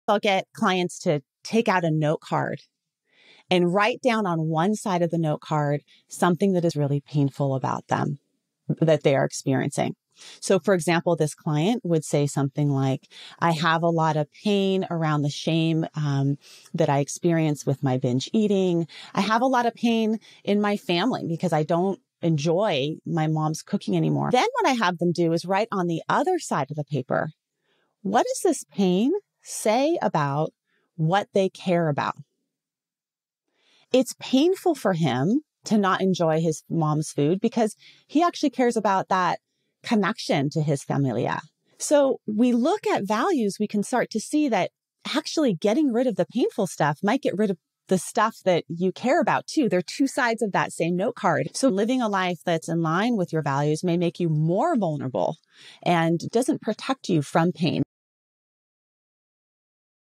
ACT Exercise: Pain and Values Note Card
Discover the deeper meanings behind your pain and learn how to align it with your values. Turn your struggles into strengths: all you need is a note card.

Learn more about ACT for Binge Eating and Anxiety and earn 1.25 CE credits